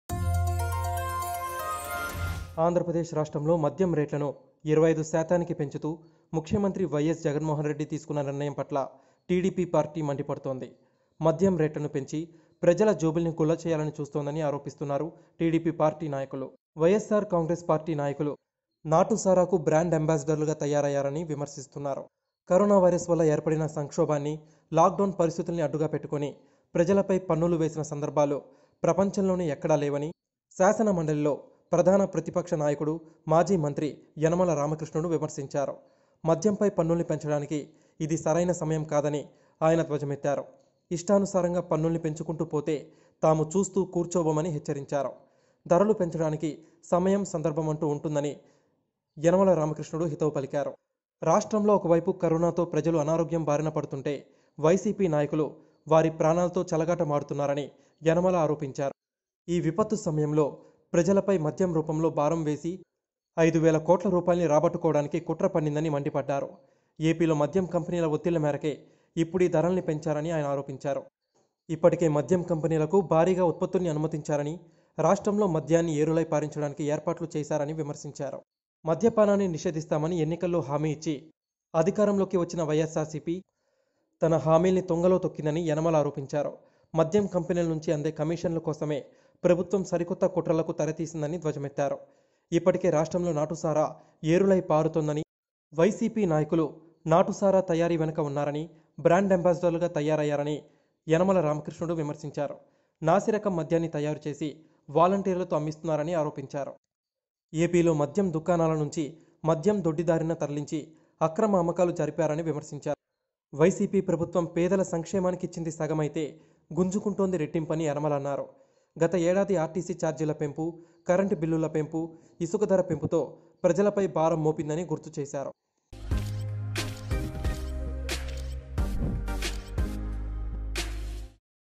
TDP Criticizes YSRCP Over Liquor Rates Hike | ప్రజల జేబులు గుల్ల చేస్తున్నారు..!! | Oneindia Telugu
Telugu Desam Party senior leader and former finance minister of Andhra Pradesh Yanamala Ramakrishnudu have criticised the Government of Andhra Pradesh led by YS Jagan Mohan Reddy on hiking the liquor rates as 25 per cent, which is sales began on Monday.

రాష్ట్రంలో మద్యం రేట్లను 25 శాతానికి పెంచుతూ ముఖ్యమంత్రి వైఎస్ జగన్మోహన్ రెడ్డి తీసుకున్న నిర్ణయం పట్ల తెలుగుదేశం పార్టీ మండి పడుతోంది. మద్యం రేట్లను పెంచి ప్రజలను జేబులను గుల్ల చేస్తోందని ఆరోపిస్తున్నారు ఆ పార్టీ నాయకులు. జగన్ పాలనలో ఎలుకలు కూడా మందు తాగడానికి అలవాటు పడ్డాయని ఎద్దేవా చేస్తున్నారు. వైఎస్ఆర్ కాంగ్రెస్ పార్టీ నాయకులు నాటుసారాకు బ్రాండ్ అంబాసిడర్లు తయారయ్యారని విమర్శిస్తున్నారు.